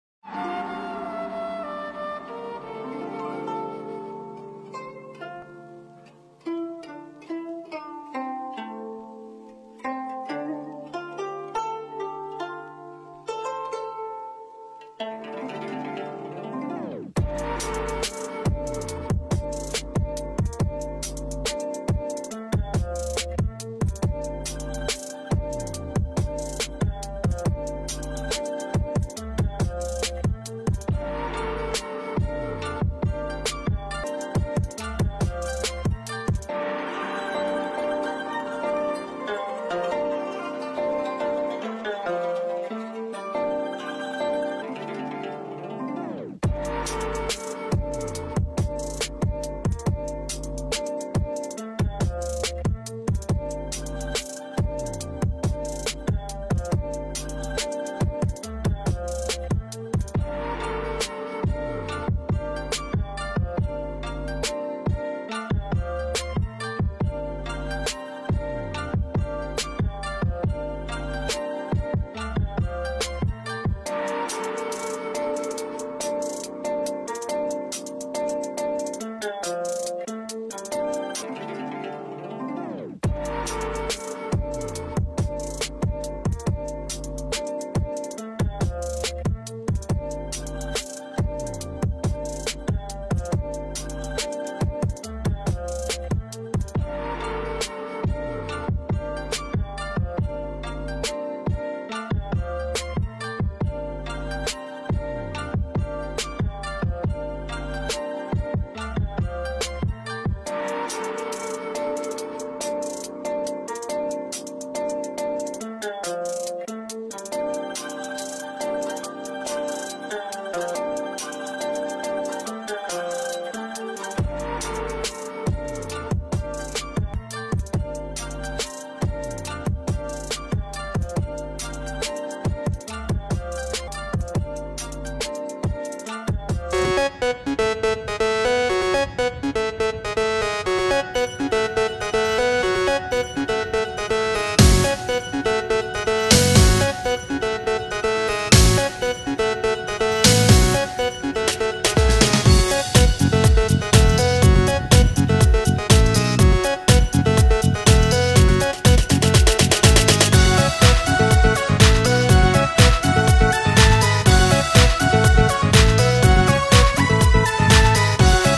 A wonderful journey of a lifetime to Lan Ha Bay, Vietnam l Blue Swimmer Ventures
Let's venture off the beaten paths like no Other in Lan Ha bay and Catba island with Blue Swimmer Outdoor Adventures. Do not travel until check out